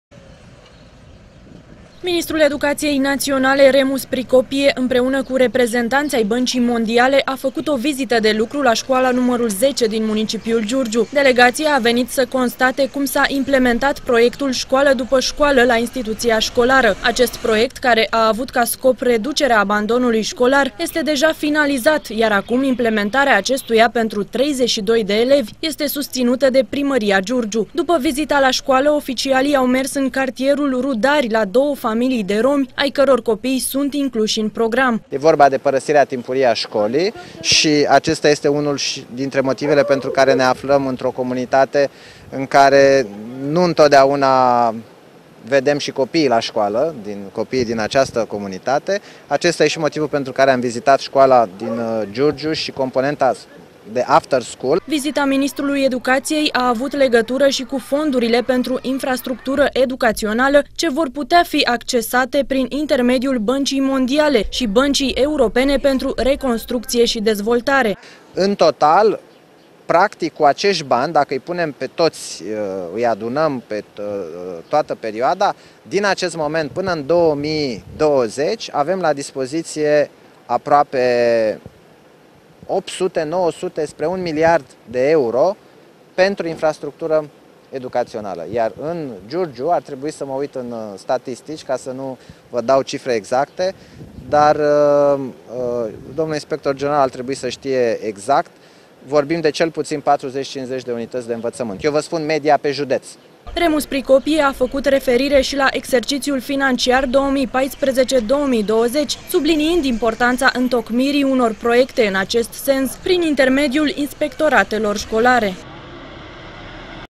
MINISTRUL EDUCATIEI LA GIURGIU
Ministrul Educației, Remus Procopie, a vizitat astăzi Școala nr. 10 din Giurgiu, pentru a vedea modul în care a fost implementat programul de tip after school "Școală după școală". Proiectul a avut ca scop prevenirea abandonului școlar. Însoțit de vicepreședintele Băncii Mondiale, oficialul a vizitat și două familii de romi ai căror copii sunt incluși în program.